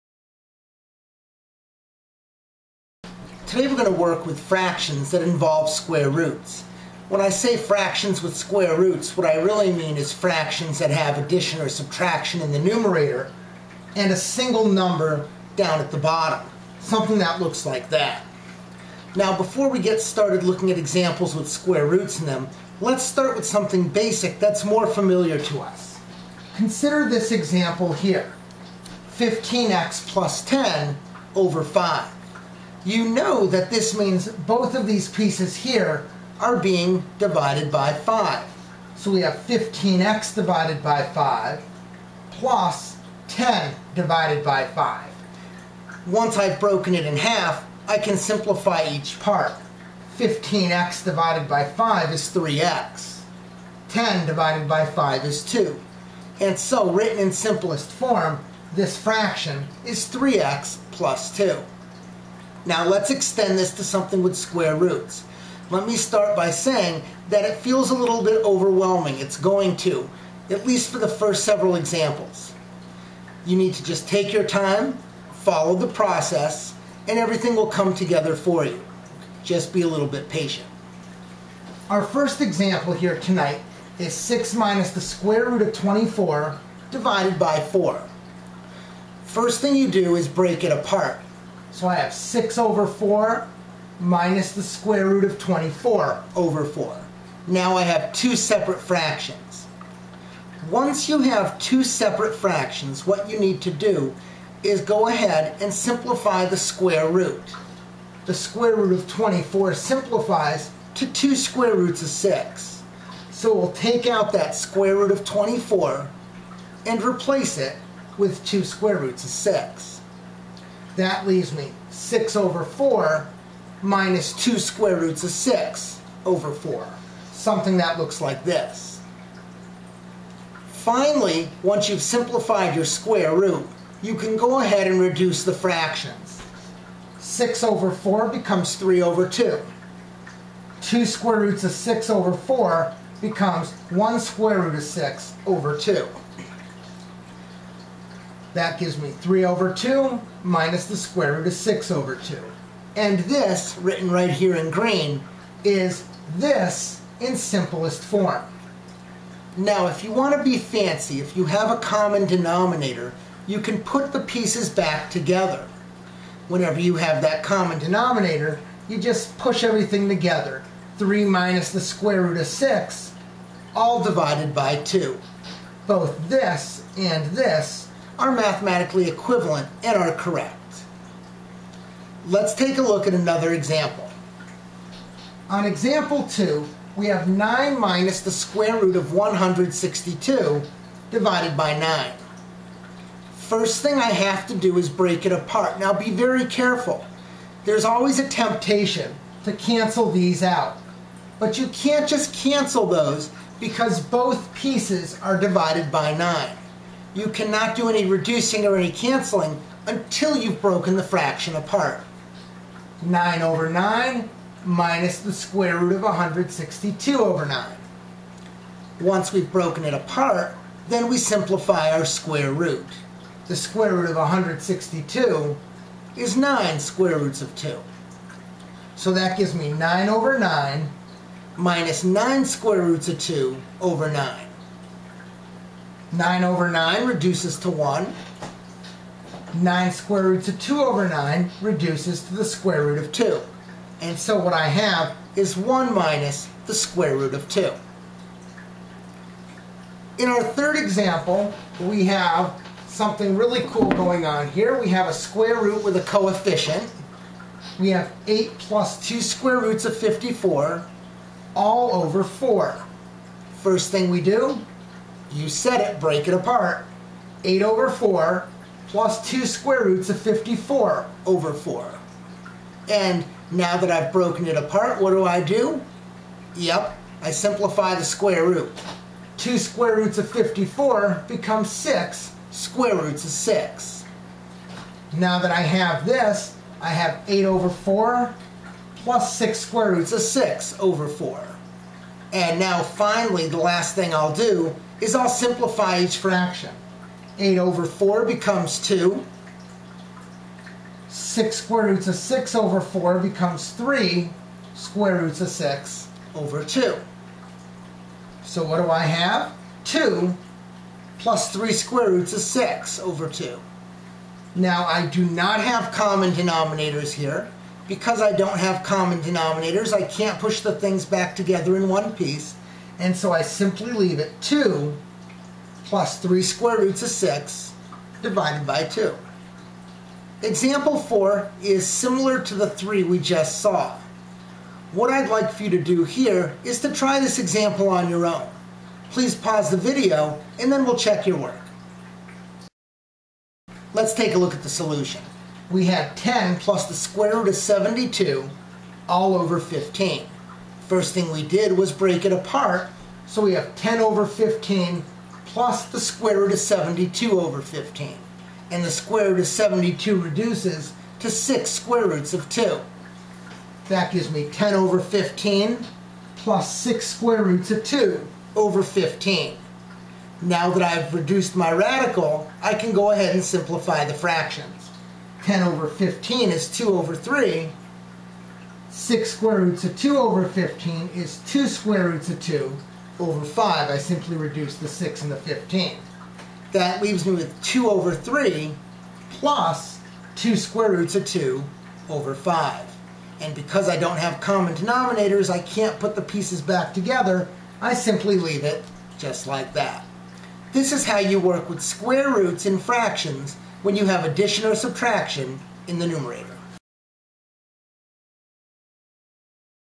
Fractions with Square Roots
How do you simplify fractions containing square roots?  Learn more in this video.